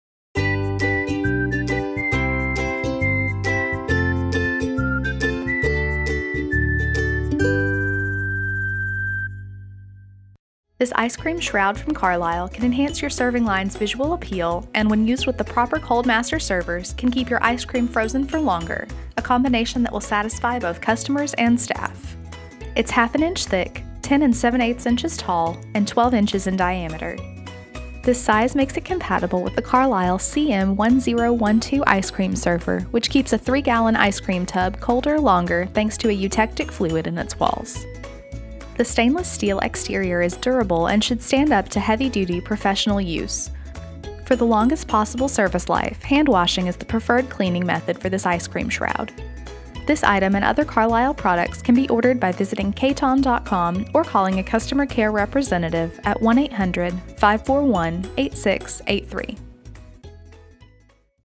Carlisle Ice Cream Shroud
The Carlisle 38655 ice cream shroud can be used to enhance your ice cream presentation and keep your product frozen for longer. For more information on this Carlisle product and the other products we offer, call a customer care representative at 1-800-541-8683